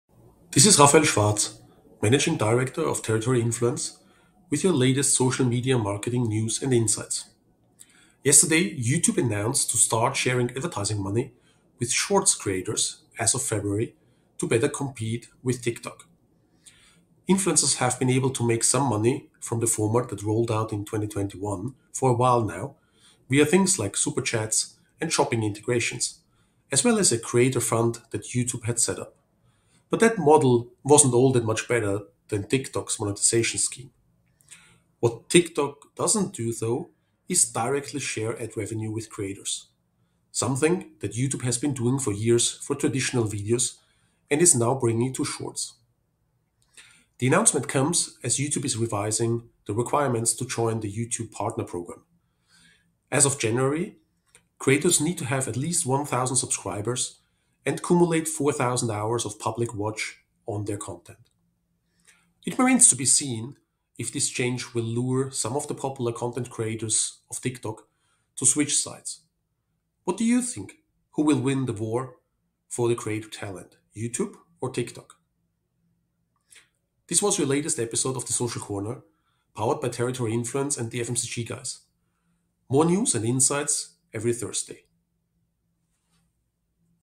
The Social Corner Ep. 5: Youtube vs TikTok
We are back with another episode of TheSocialCorner, hosted by Rafael Schwarz , who is looking at the latest developments at YouTube and TikTok and their battle for the most popular content creators.

Which platform do you favor and who do you think has the better creators at the moment?